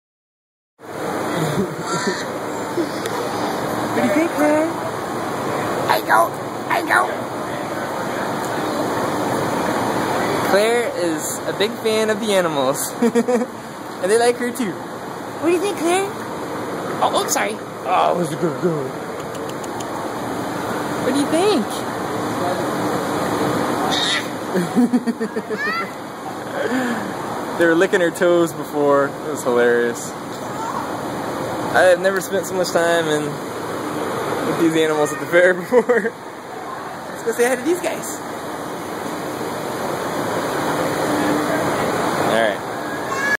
Fun at the Johnson Co Fair
I decided to take Claire to the fair on the last day, what a great decision!  So family friendly, I'm not sure who had more fun, her or me.  Well, the answer is definitely her.  But I was having a blast watching her get excited and laugh at all the animals!  She would say hi to all the cows and moo softly, the pigs made her squeal with delight, we had fun crowing along with the roosters, and here you see her with some of the goats, so great.  We are going to become fair regulars!